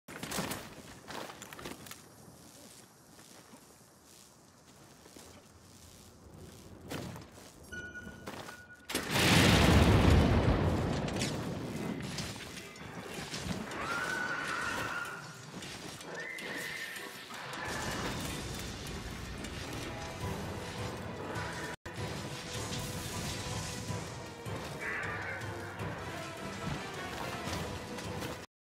Kirin go boooom!
Sometimes it's just so satisfying to blow this lightning unicorn the $@!% up 😁 but then he walks out of the smoke like 😈 and your like 😲😭